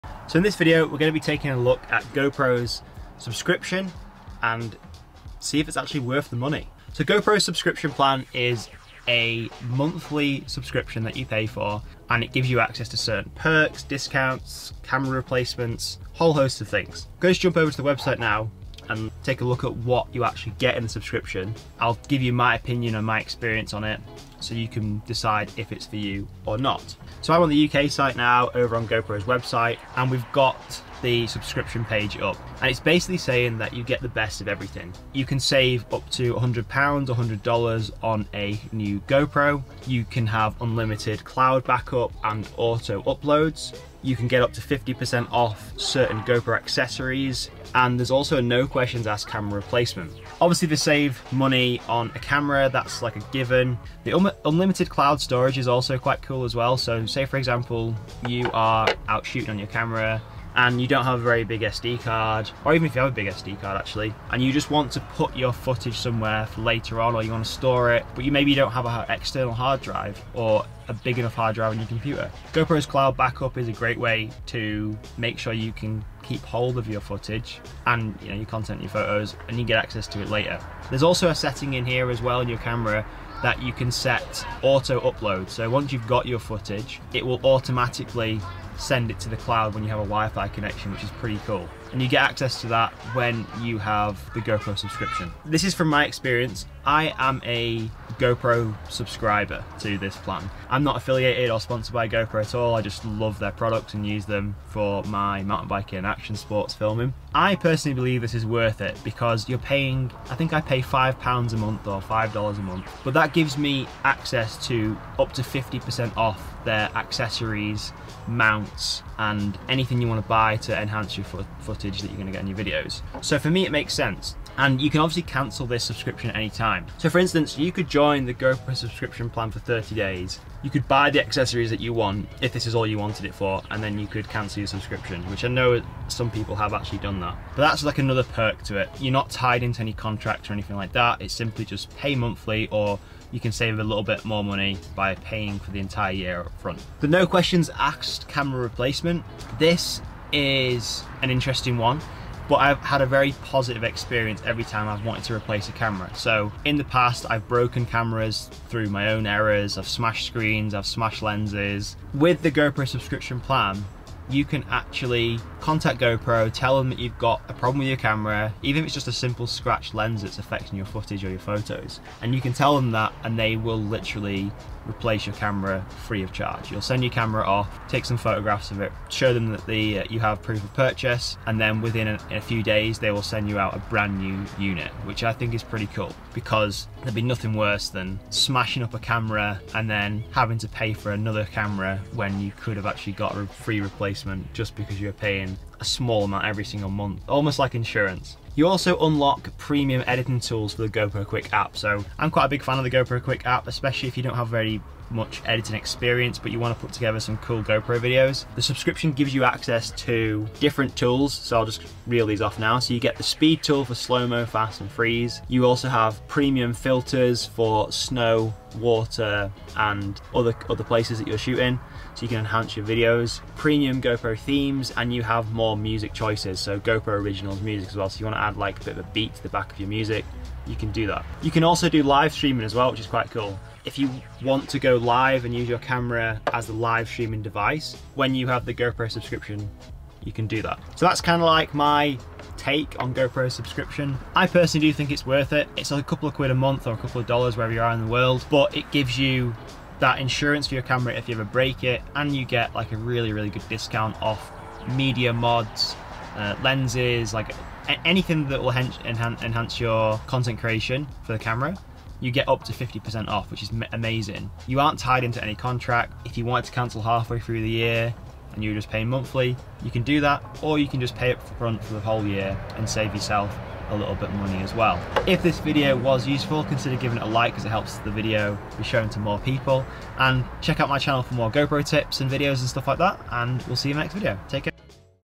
GoPro Subscription: Honest Thoughts & Should You Get It?
Here's my thoughts on the GoPro subscription, and whether it's actually worth it.